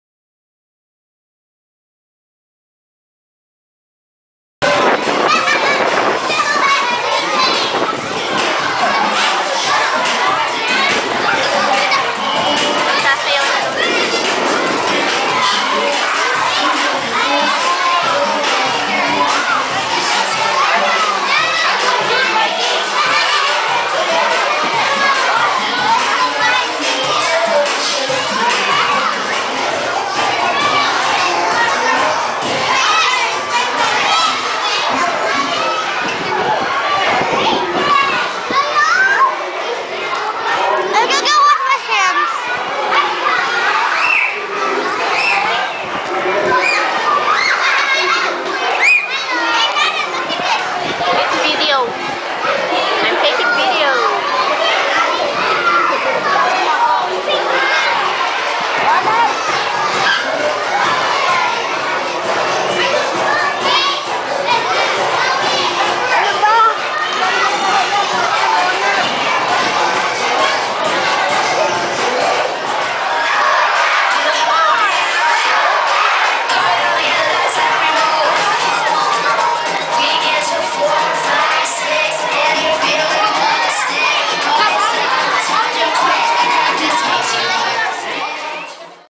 Wettengel Elem. School Guam
Grade K to 2nd Grade

Dance at the pavillion after a whole week of SAT test...